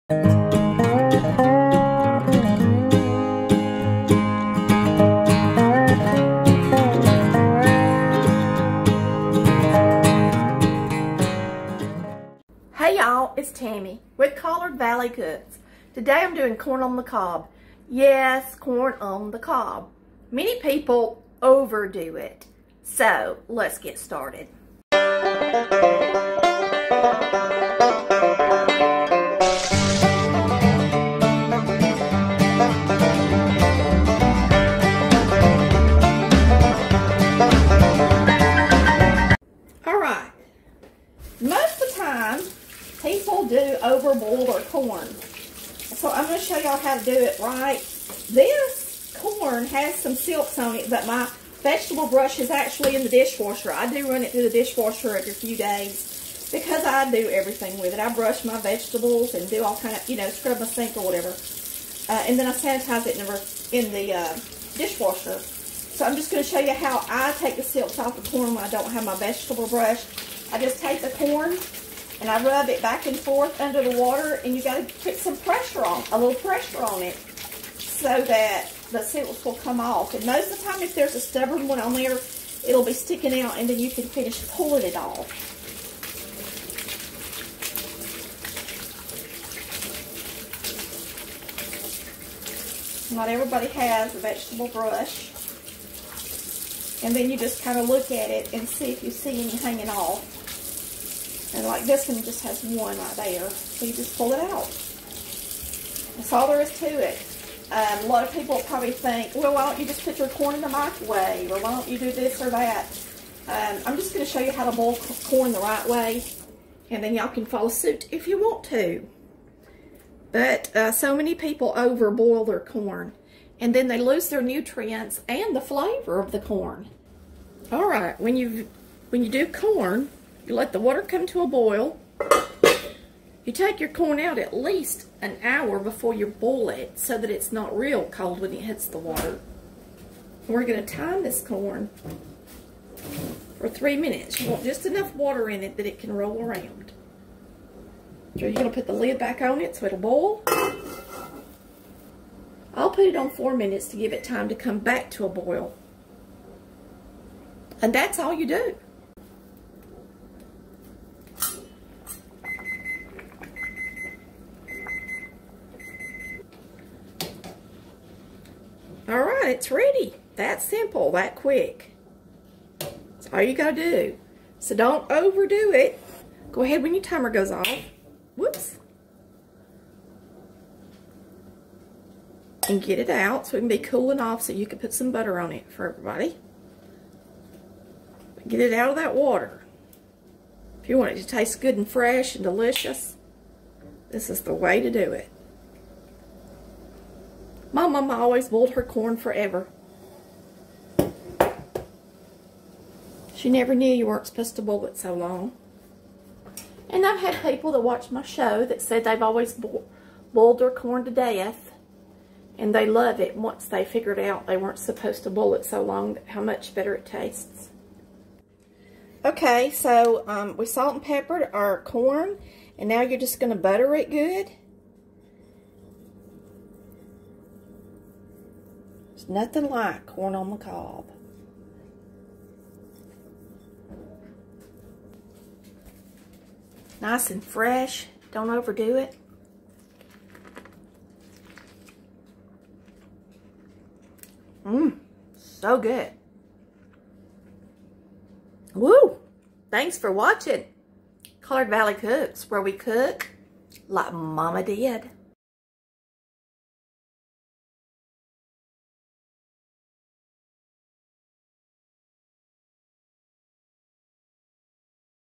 The Correct Way to Make Corn on the Cob, Best Southern Cooking
Today I am showing you how to boil your corn.  You should put it in when it is room temperature so it doesn't cool down your boiling water. Bring water to a boil and add corn.  Boil for 4 minutes.  Take out of the water, salt and pepper and butter the corn.

PAYEER  P13395599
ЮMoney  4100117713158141
VISA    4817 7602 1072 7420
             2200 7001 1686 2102